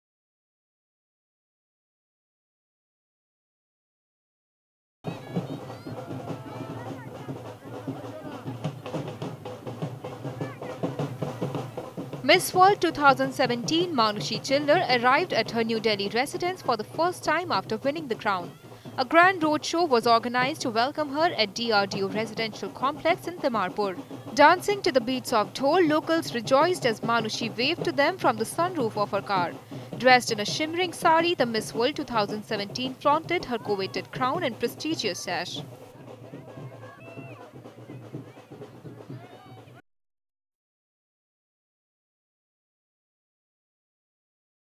Manushi Chhillar arrives at her New Delhi residence after winning the crown
New Delhi, Dec 03 (ANI): Miss World 2017 Manushi Chhillar arrived at her New Delhi residence for the first time after winning the crown. A grand road show was organised to welcome her at DRDO residential complex in Timarpur. Dancing to the beats of dhol, locals rejoiced as Manushi waved to them from the sunroof of her car. Dressed in a shimmering saree, the Miss World 2017 flaunted her coveted crown and prestigious sash.